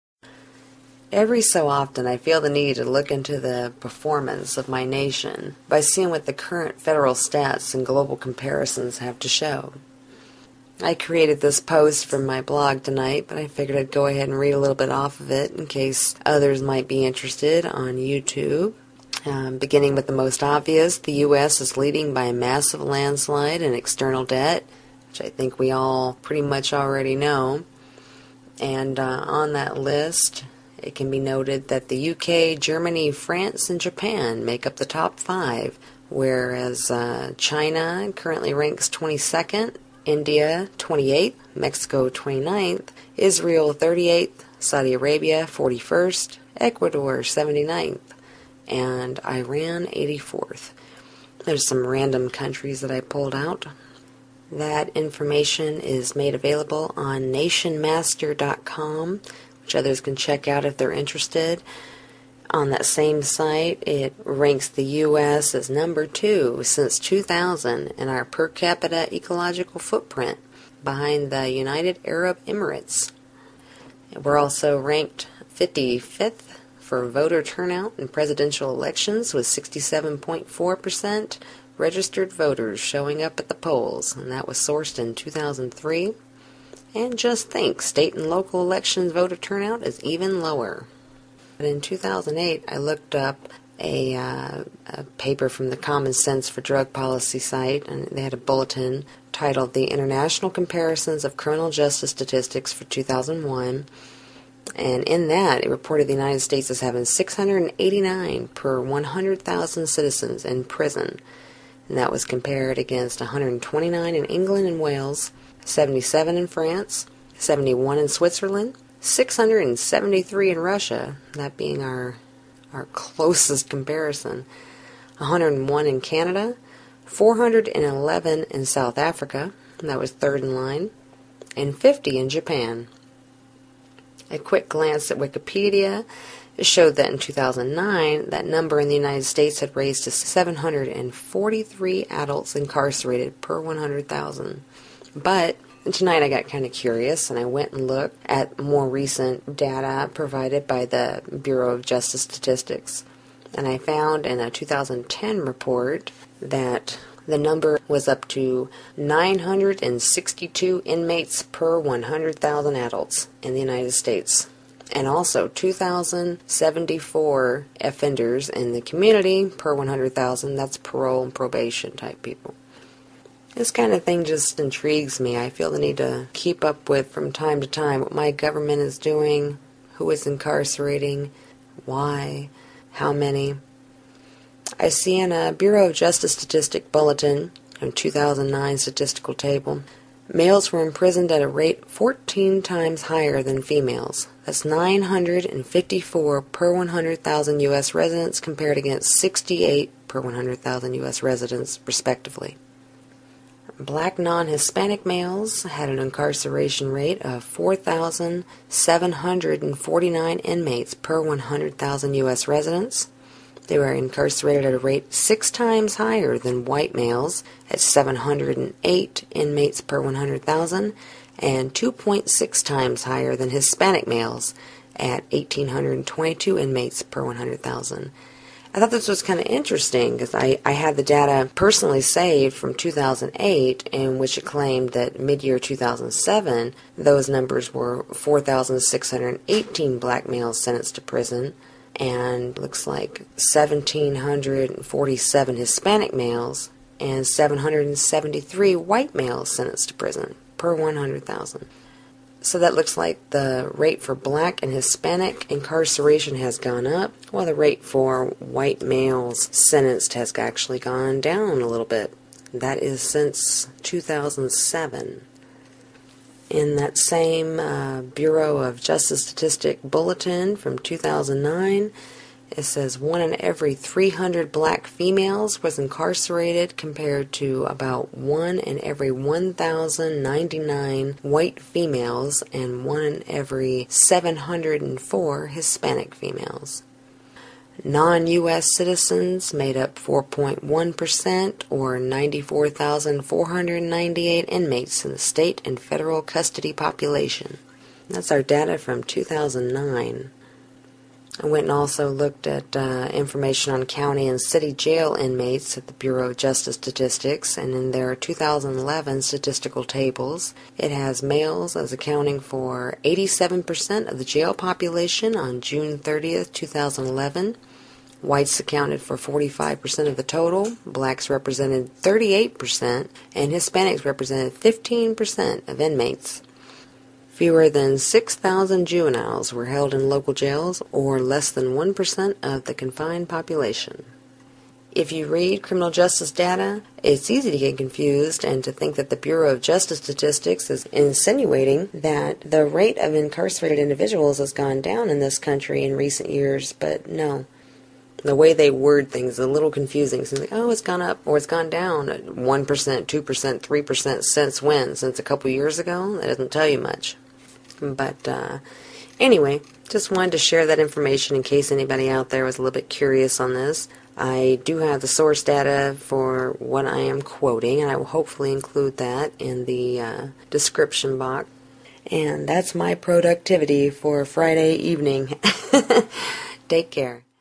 A glimpse into U.S. ranking and stats (including prisoner data)
Just sharing a little information pertaining to my country and its ranking when compared against other countries in terms of external debt and number of citizens imprisoned.